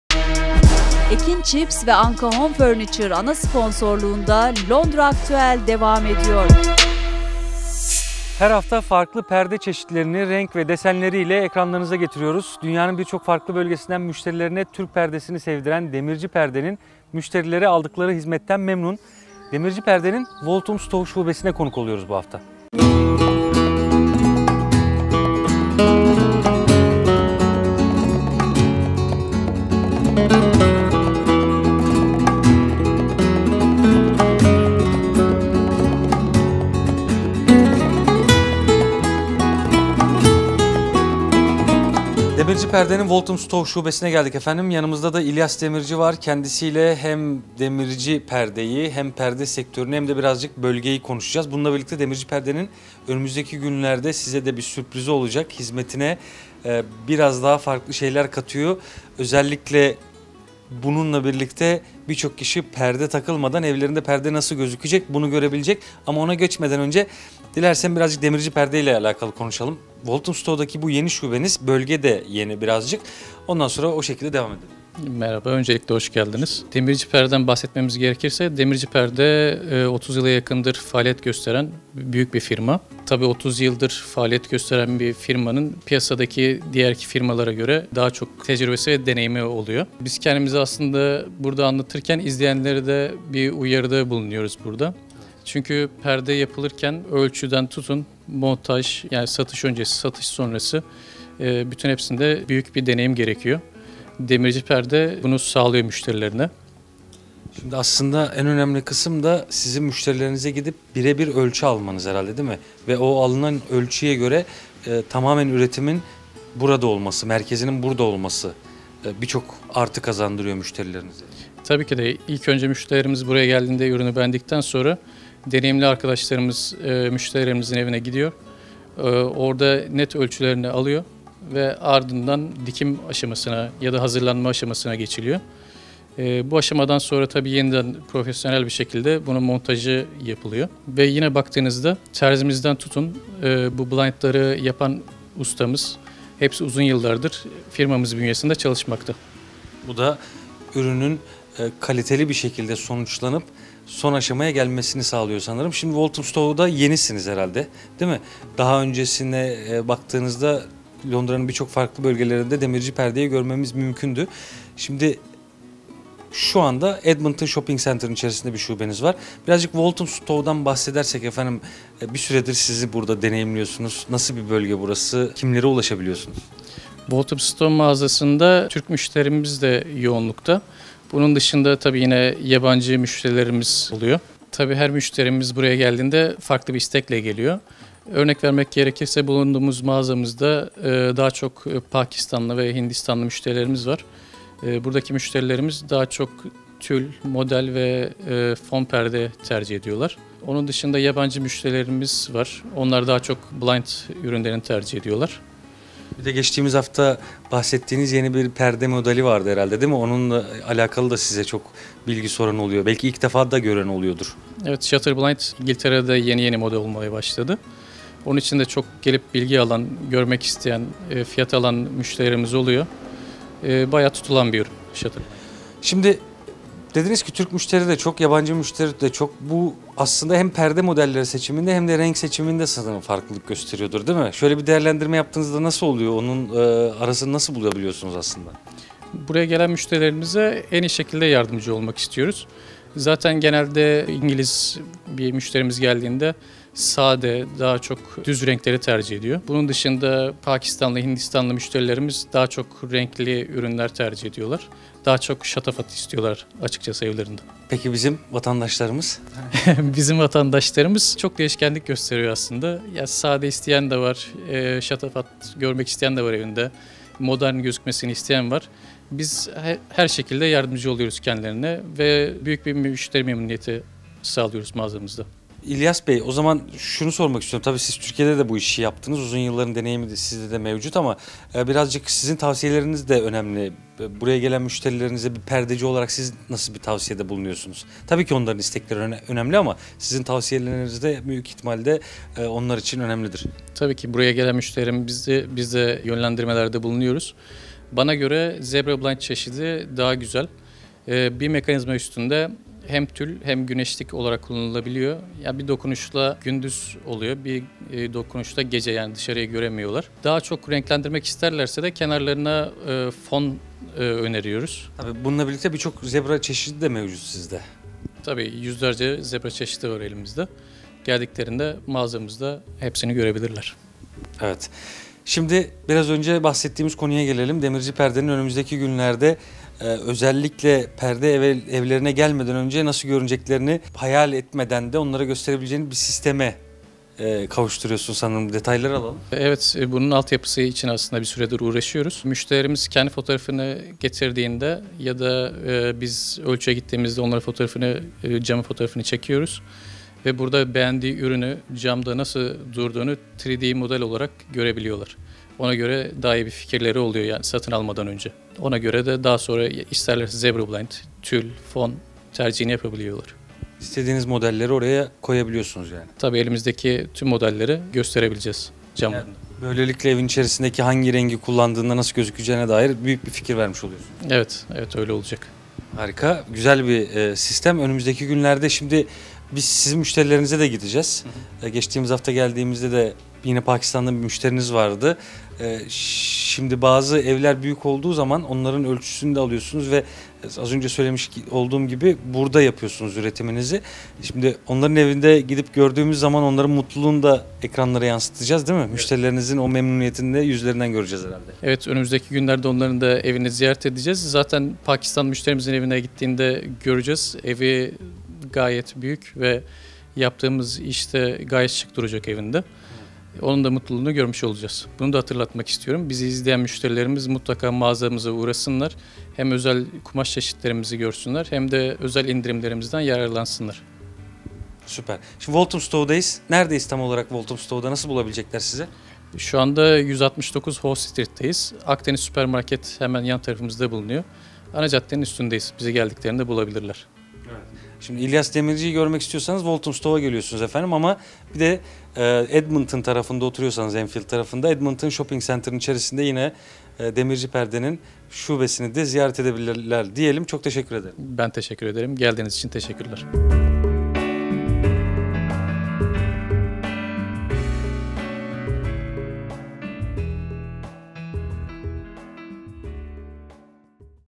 Perde Sektöründe Öncü Bir Firma / Demirci Perde / Londra Aktüel 188
Her hafta farklı perde çeşitlerini, renk ve desenleriyle ekranlara getiriyoruz. Dünyanın birçok farklı bölgesinden müşterilerine Türk perdesini sevdiren demirci perdenin müşterileri aldıkları hizmetten
memnun. Demirci Perde’nin Walthamstow şubesine konuk oluyoruz bu hafta.